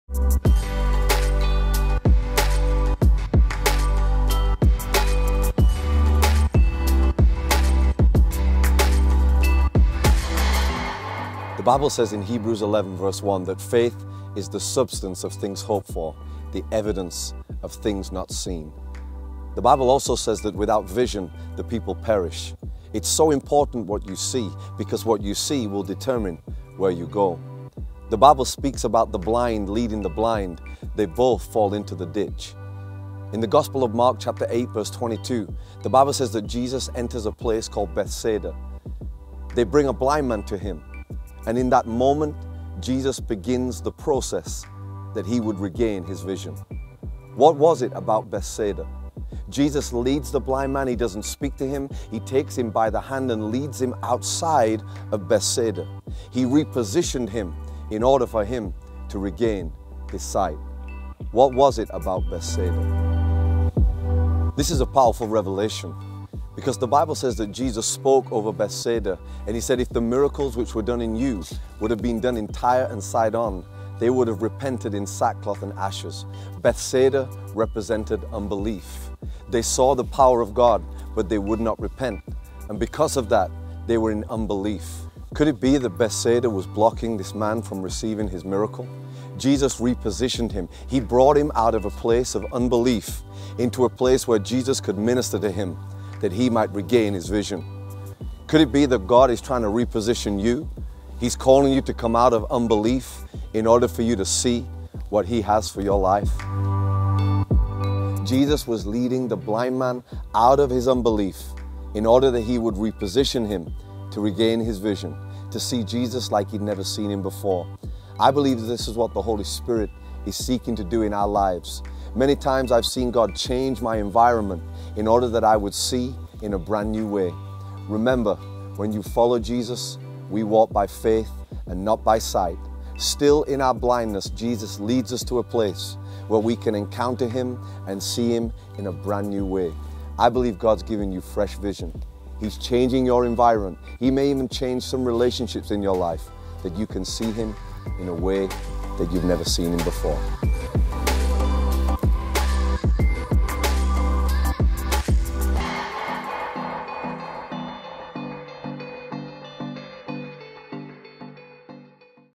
How To Deal With Unbelief - Faith Talks - Nathan Morris
In this Faith Talks episode, Evangelist Nathan Morris discusses the topic of Dealing With Unbelief. We hope this video encourages you and answers some questions you have been having about your faith.